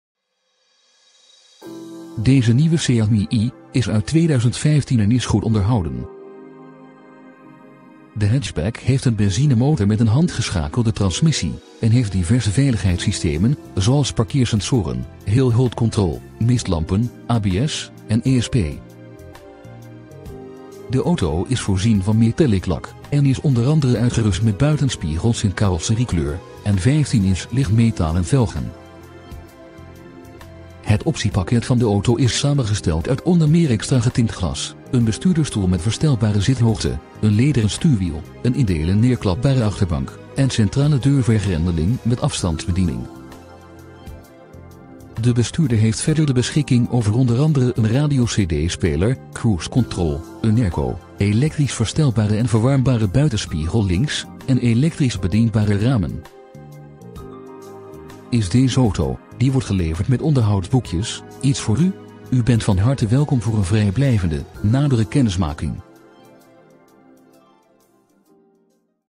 Seat Mii 1.0 Sport Connect | PDC | CRUISE | AIRCO
1 km
  Prijs:     € 11.495
  Merk:     Seat
  Model:     Mii
  Type:     1.0 Sport Connect | PDC | CRUISE | AIRCO
  Carrosserievorm:     Hatchback (5 drs)
  Bouwjaar:     2015
  Transmissie:     handgeschakeld
  Kleur:     Nachtblauw metallic
  BTW/Marge:     btw
  Brandstof:     benzine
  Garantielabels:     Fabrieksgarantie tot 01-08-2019

Accessoires
* 3e remlicht
* ABS
* Achterbank neerklapbaar in delen
* Airbags
* Airco
* Audio installatie
* Bestuurdersstoel met verstelbare zithoogte
* Boordcomputer
* Cruise control
* ESP
* Metallic lak
* Mistlampen voor
* Multimedia voorbereiding
* Parkeersensoren achter
* Radio/CD speler
* Ramen voor elektrisch bedienbaar
* Side-airbags voor
* Stuur bekleed met leder
* Stuurbekrachtiging

Afleverkosten: € 995
Boekjes: Aanwezig

De nieuwe Seat Mii Sport Connect, zeer compleet inclusief Lm-velgen, getint glas, PDC, Cruise control etc etc Nu geheel Rijklaarprijs 12.454,- !!

Aanbieder
Heron Enkhuizen
De Dolfijn 1
1601ME ENKHUIZEN